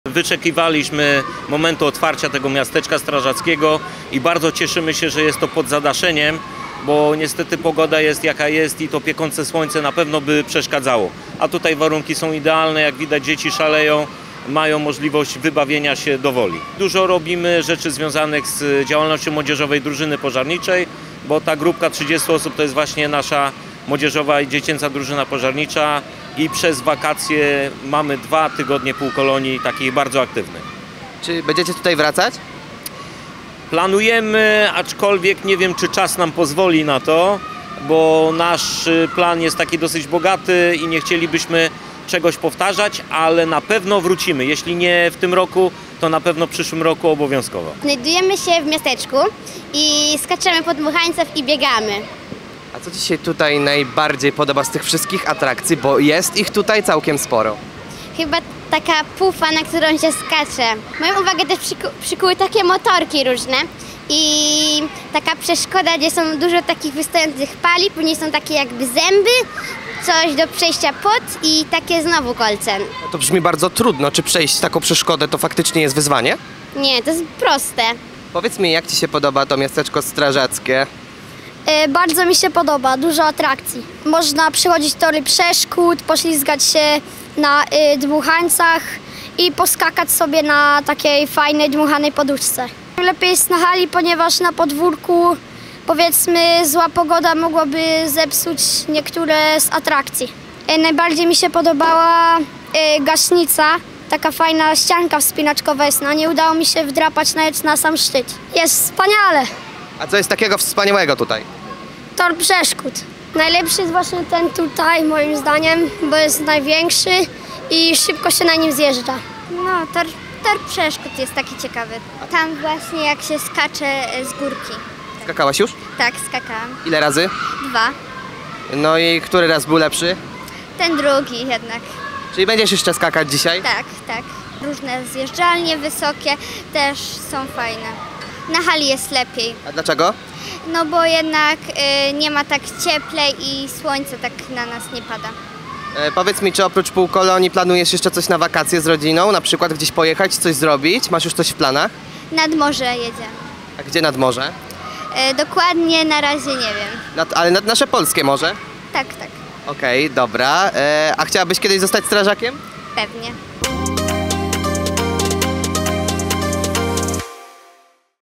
Miasteczko strażackie ponownie zawitało do CWK
w Centrum Wystawienniczo - kongresowego w Opolu ponownie pojawiło się Miasteczko Strażackie. W klimatyzowanej hali CWK znajdują się kolorowe dmuchańce, tory przeszkód, nadmuchiwana ścianka wspinaczkowa oraz zjeżdżalnie. Obiekt cieszy się zainteresowaniem nie tylko wśród mieszkańców Opola a również do miasteczka przyjeżdżają półkolonie z całego regionu. Obiekt jest czynny przez cały tydzień w godzinach 10:00 - 19:00. Atrakcja będzie dostępna jeszcze przez najbliższy tydzień.